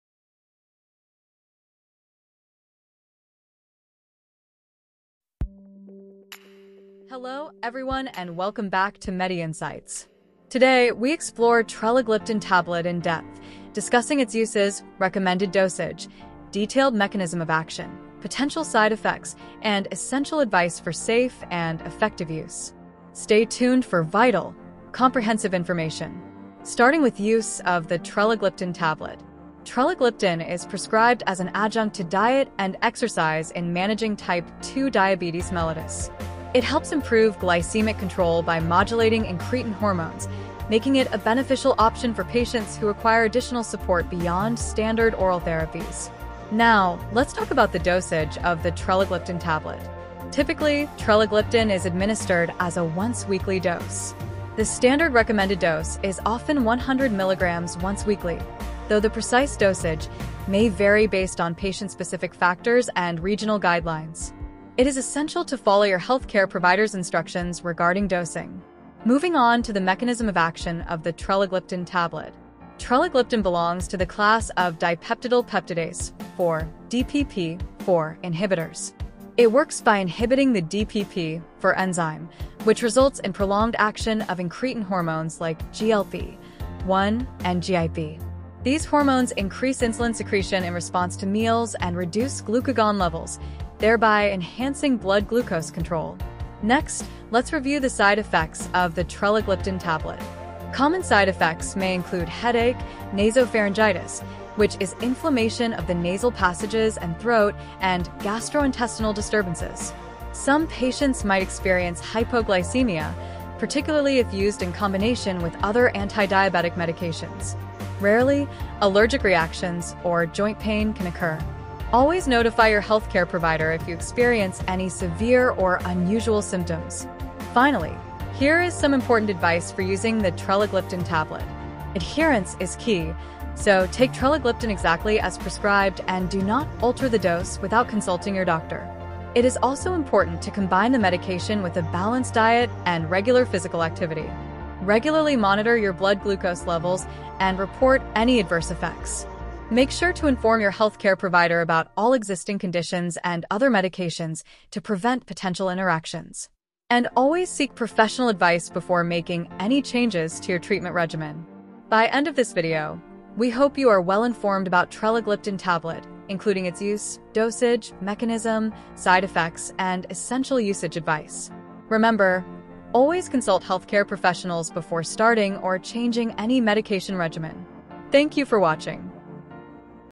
Trelagliptin Tablet: Uses, Dosage, Mechanism, Side Effcets & Safety Guide | MediInsights
Welcome to MediInsights – Unlocking The Wonders of Wellness. In today’s video, we dive deep into everything you need to know about the Trelagliptin Tablet. Are you searching for a comprehensive guide on this innovative medication used in managing type 2 diabetes? Look no further! Our detailed discussion covers the use of Trelagliptin Tablet as an adjunct to diet and exercise, its recommended dosage, the intricate mechanism of action, potential side effects, and crucial advice for safe and effective use.

Trelagliptin Tablet is a breakthrough medication specifically prescribed for patients with type 2 diabetes mellitus who require enhanced glycemic control beyond standard therapies. By modulating incretin hormones, this medicine helps improve blood sugar regulation and overall metabolic health. In this video, we explain how Trelagliptin functions as a DPP-4 inhibitor, effectively prolonging the action of key hormones such as GLP-1 and GIP. This results in increased insulin secretion in response to meals and reduced glucagon levels, paving the way for improved glycemic stability.

We take an in-depth look at the recommended dosage of Trelagliptin Tablet. Typically administered as a once-weekly dose – with 100 mg being the standard in many cases – this medication’s dosing can vary based on individual patient factors and regional clinical guidelines. Understanding and adhering to the correct dosage is vital to maximize therapeutic benefits while minimizing the risk of side effects. Our video emphasizes the importance of following your healthcare provider’s instructions to ensure optimal outcomes and safe use of the medication.

The mechanism of action of Trelagliptin is another focal point of our presentation. As a member of the dipeptidyl peptidase-4 (DPP-4) inhibitor class, Trelagliptin works by targeting and inhibiting the DPP-4 enzyme. This inhibition allows for a sustained increase in the levels and action of incretin hormones, which are crucial in managing postprandial blood glucose spikes. With improved insulin secretion and reduced glucagon levels, patients can experience a more controlled and steady blood sugar profile.

No medication is without potential side effects. In our video, we discuss the common side effects associated with Trelagliptin Tablet, including headache, nasopharyngitis (inflammation of the nasal passages and throat), and gastrointestinal disturbances. We also highlight the possibility of hypoglycemia, particularly when Trelagliptin is used in combination with other antidiabetic medications. Additionally, we advise viewers to promptly consult their healthcare provider should they experience any severe or unusual symptoms.

Finally, our comprehensive guide offers important advice for the safe use of Trelagliptin Tablet. We stress the need for adherence to prescribed doses, the integration of a balanced diet and regular physical activity, continuous blood glucose monitoring, and open communication with your healthcare provider regarding any changes in your medical history or treatment plan. This essential advice is designed to empower you to take control of your diabetes management and optimize your overall wellness.

Join us as we explore the evolving world of healthcare and empower you with knowledge to enhance your quality of life.

CHAPTERS:
0:00 :INTRO
0:27 :USE OF TRELAGLIPTIN TABLET
0:46 :DOSAGE OF TRELAGLIPTIN TABLET
1:09 :MECHANISM OF ACTION OF TRELAGLIPTIN TABLET
1:37 :SIDE EFFECTS OF TRELAGLIPTIN TABLET
2:08 :IMPORTANT ADVICES FOR USING TRELAGLIPTIN TABLET